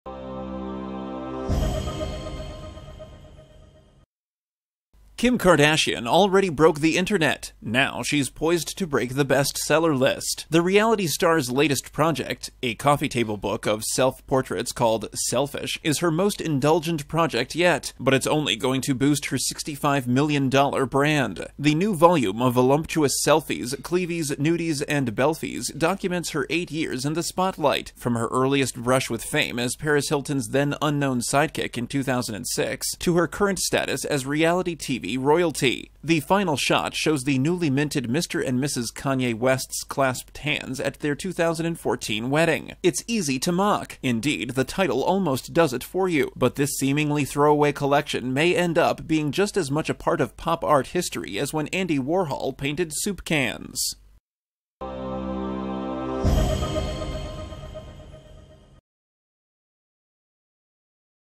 'Selfish,' Kim Kardashian's Book With 300 Selfies, Bound for a Coffee Table Near You
Kim Kardashian already broke the Internet. Now she’s poised to break the best-seller lists. The reality star’s latest project — a coffee-table book of self-portraits, called “Selfish” — is her most indulgent project yet, but it’s only going to boost her $65 million brand. The new volume of voluptuous selfies, cleavies, nudies and belfies documents her eight years in the spotlight — from her earliest brush with fame as Paris Hilton’s then-unknown sidekick in 2006 to her current status as reality TV royalty. The final shot shows the newly minted Mr. and Mrs. Kanye West’s clasped hands at their 2014 wedding. It’s easy to mock — indeed, the title almost does it for you — but this seemingly throwaway collection may end up being just as much a part of pop-art history as when Andy Warhol painted soup cans.